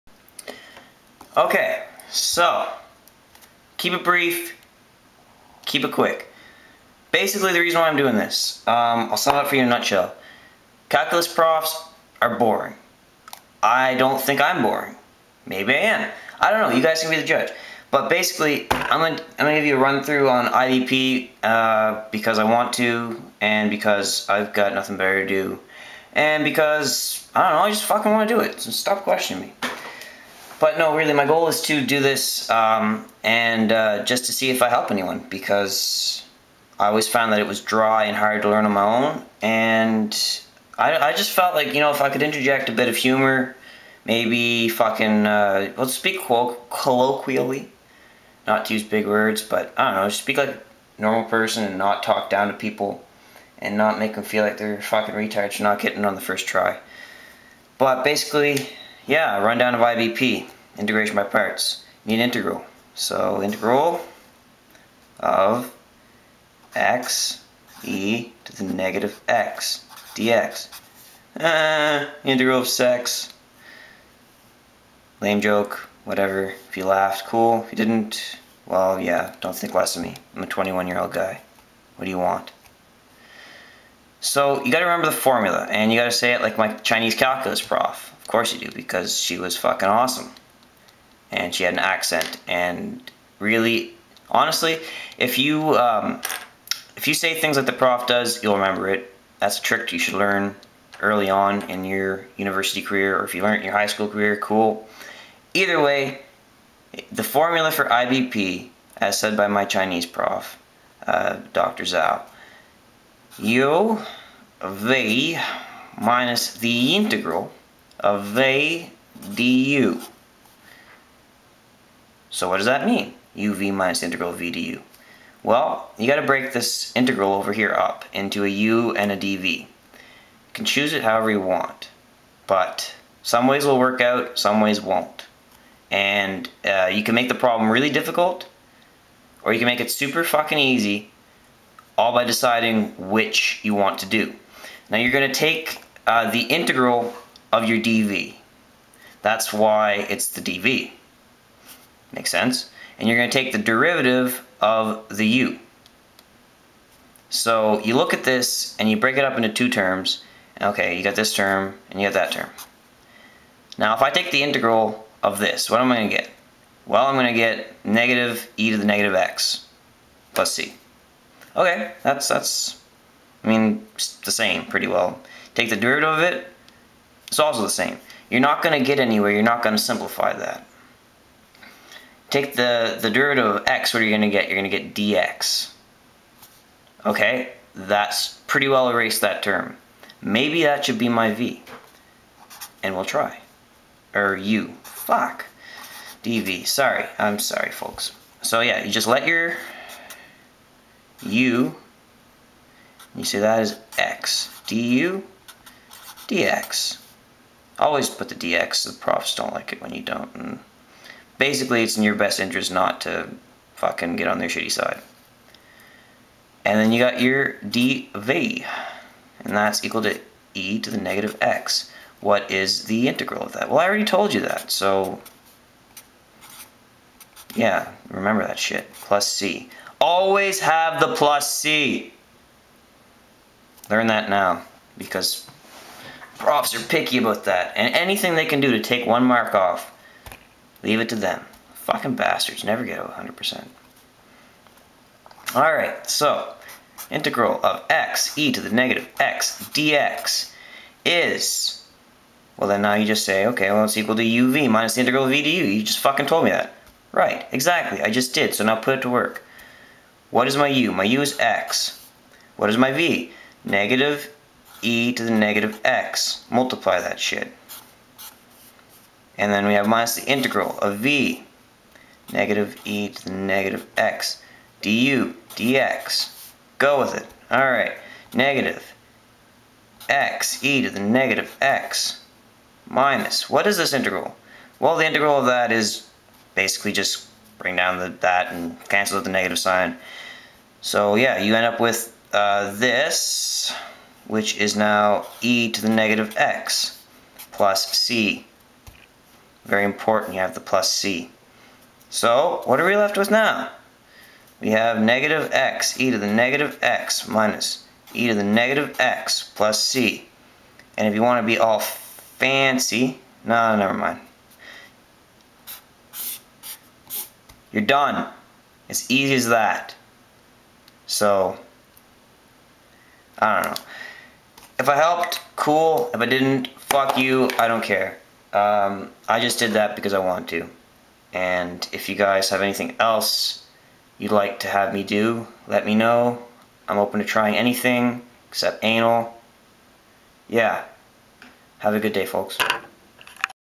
Integration By Parts LEARN IT!!!!
So here's my crack at making a non boring calculus video. Let me know if it helped.